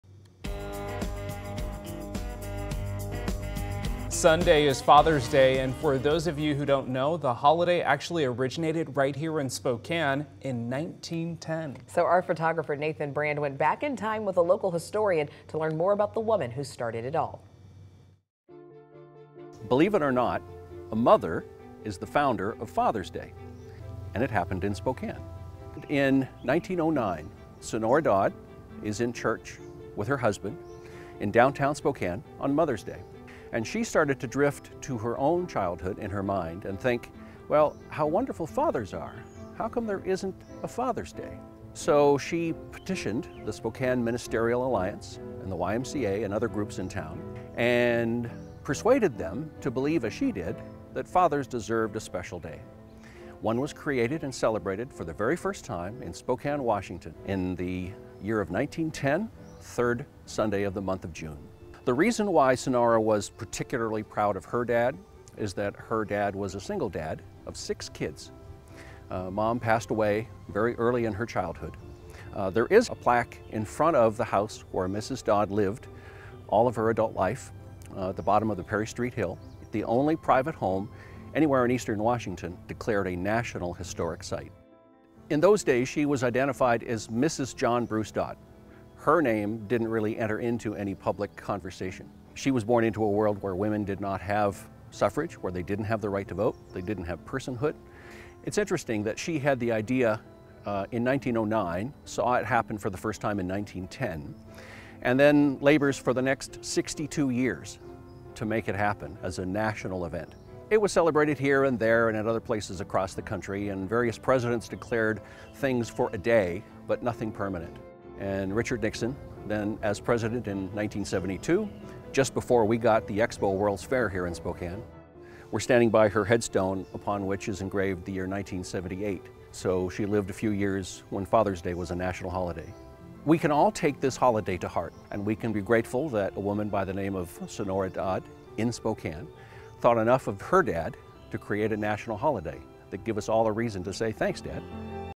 History of Father's Day: Founded in Spokane
Father's Day originated in Spokane in 1910. Here's more about the woman behind it and how it became a national holiday.